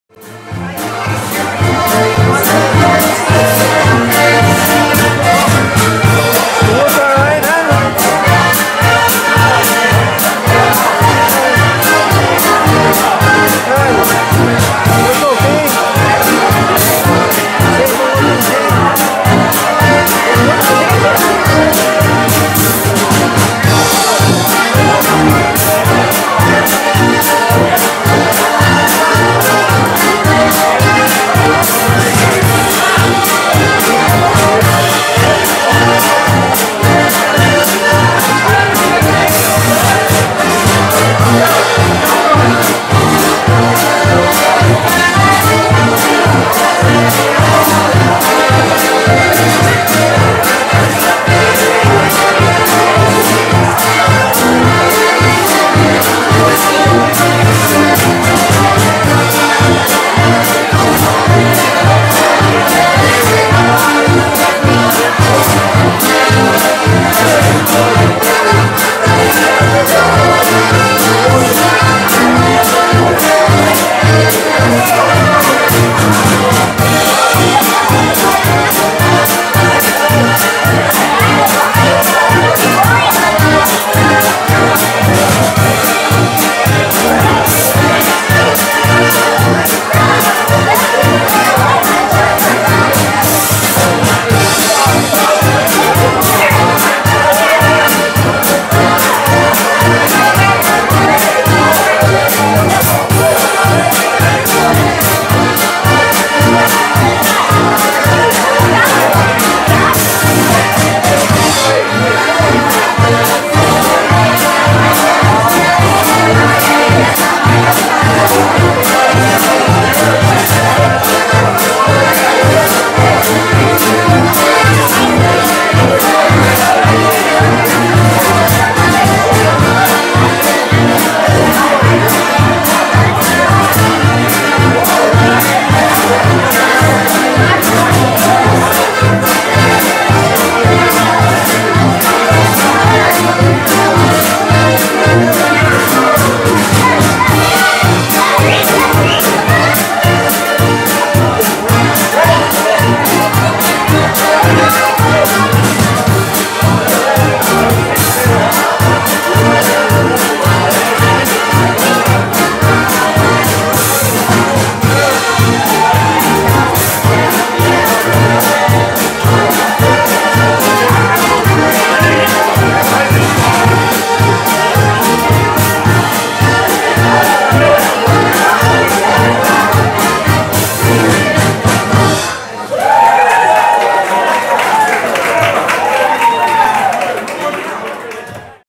BUTTON BOX JAMMING CELEBRATING JIM & BONNIE'S 50TH ANNIVERSARY "HAPPY TIMES"
Celebrating JIM and BONNIE'S 50TH Wedding Anniversary at American Legion Post Milan, Mi. August 15, 2017 A large crowd enjoying FINE food  GREAT music from BUTTON BOX JAMMING & dancing for a SUPER CELEBRATION. THANK YOU JIM & BONNIE